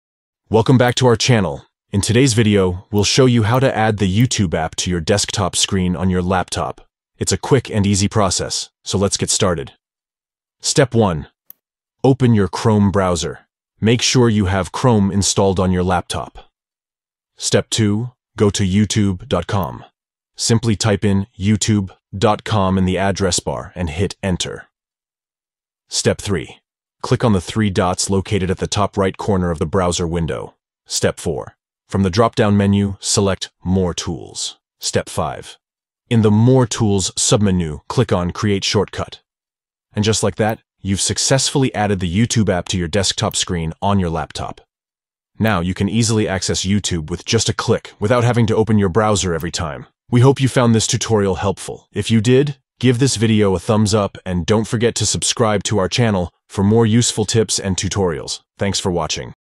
How to Add YouTube App on Desktop Screen Laptop
Want quick and easy access to YouTube on your laptop? This step-by-step tutorial will guide you on how to add the YouTube app to your desktop screen. Follow these simple steps:

1. Open your Chrome browser.
2. Go to youtube.com.
3. Click on the three dots at the top right corner.
4. Select "More tools" from the dropdown menu.
5. Click on "Create shortcut".

By adding the YouTube app to your laptop's desktop screen, you can conveniently launch YouTube with just a click, eliminating the need to open your browser every time. Watch this video to learn how to add the YouTube app to your desktop screen on your laptop.